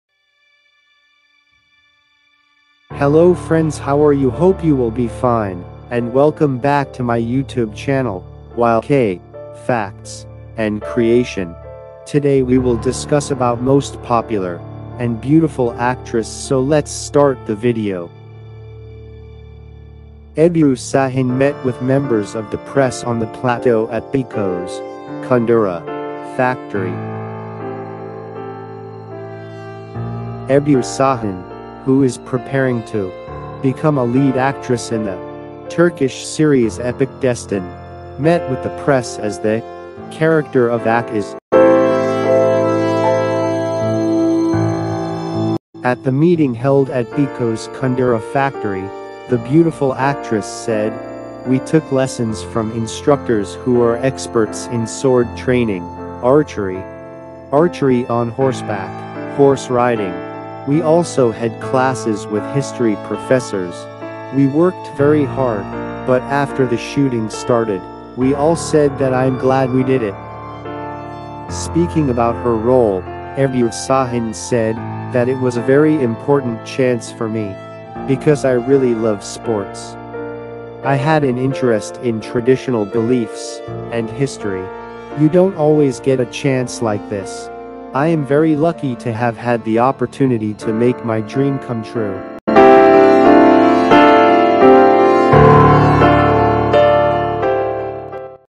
Ebru Sahin: Such a Chance Does Not Come As Always
Ebru Sahin met with members of the press on the plateau at Beykoz Kundura Factory.

Ebru Sahin, who is preparing to become a lead actress in the Turkish series Epic (Destan), met with the press as the character of 'Akkiz'.

At the meeting held at Beykoz Kundura Factory, the beautiful actress said "We took lessons from instructors who are experts in sword training, archery, archery on horseback, horse riding. We also had classes with history professors. We worked very hard, but after the shooting started, we all said that "I'm glad we did it."

Speaking about her role, Ebru Sahin said that "It was a very important chance for me because I really love sports. I had an interest in traditional beliefs and history. You don't always get a chance like this. I am very lucky to have had the opportunity to make my dream come true."

Epic (Destan) tv series will be broadcasted on ATV.

Akkiz
Ebru Sahin's boyfriend Cedi Osman also supported the beautiful actress on social media through posting "Although I cannot be there beside you on this special day, my heart and mind are always with you, darling. I hope all your dreams come true because you always deserve the best."